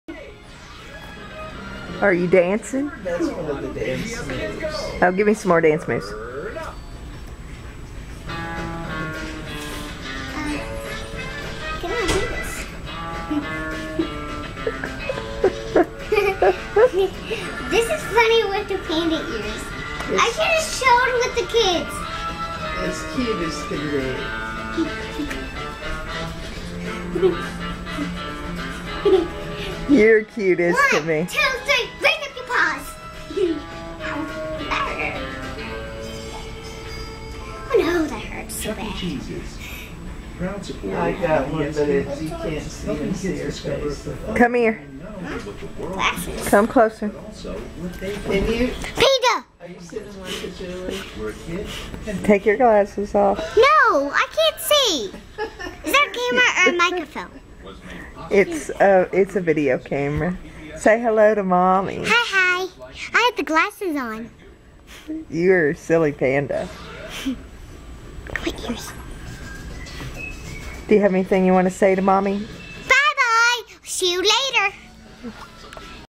Nen in her panda ears
Practicing her VBS dancing while wearing Ama's reading glasses